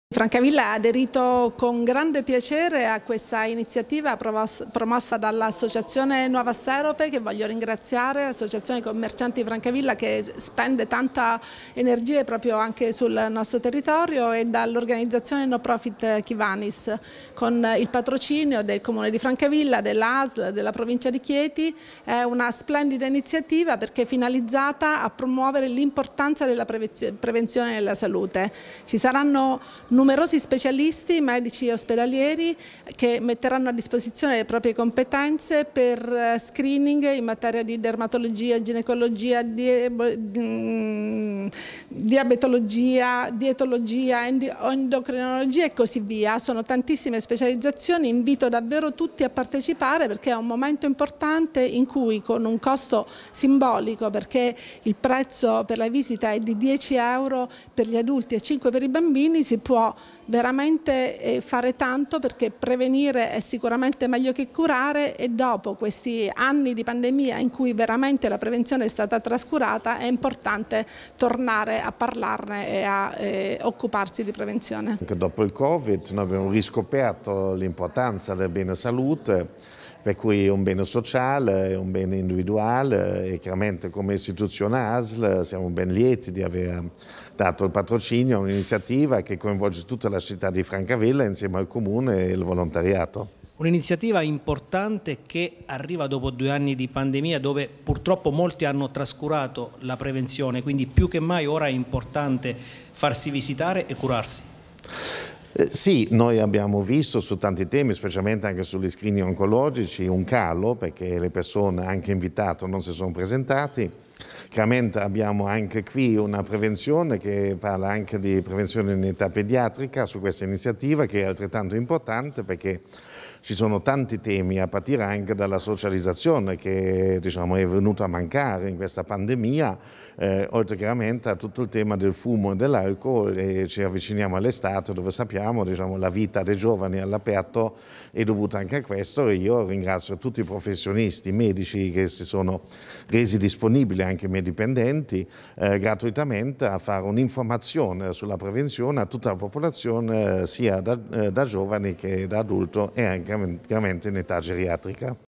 A Francavilla il 4 giugno si terrà la Giornata della Salute
L’Amministrazione Comunale di Francavilla al Mare ha presentato la Giornata della Salute, che avrà luogo ‪il 4 giugno‬ in piazza Sant'Alfonso e darà la possibilità di eseguire controlli e visite mediche  con più di quindici medici specialisti.